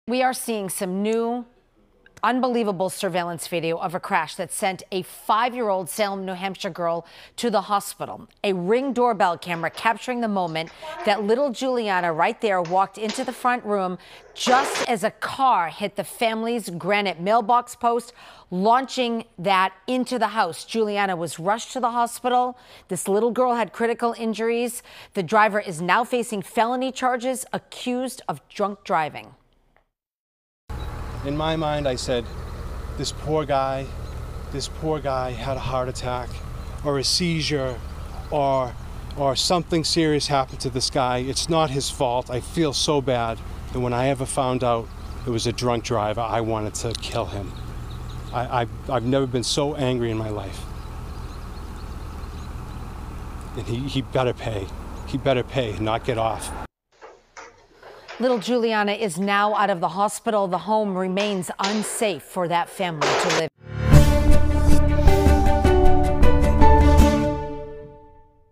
Home surveillance video shows violent New Hampshire crash that critically injured 5-year-old girl
The driver, who struck a granite mailbox post that went flying into the house, has been charged with driving under the influence.